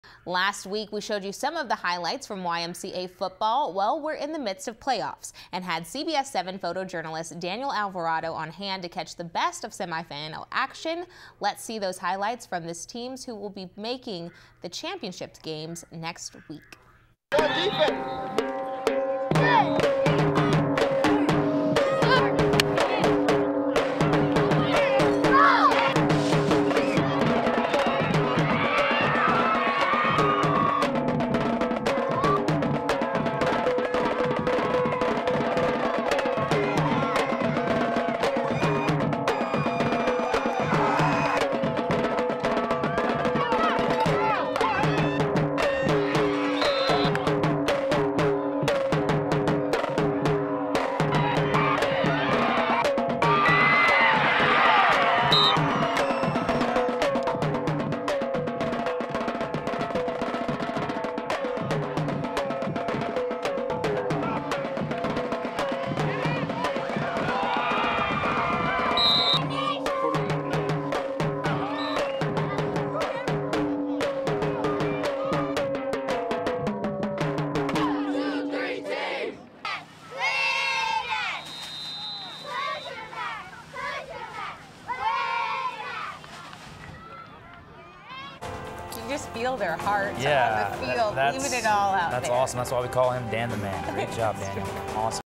YMCA Football
SemiFinal round of the 2015 YMCA Football season, featuring the Kermit Yellowjacket Band.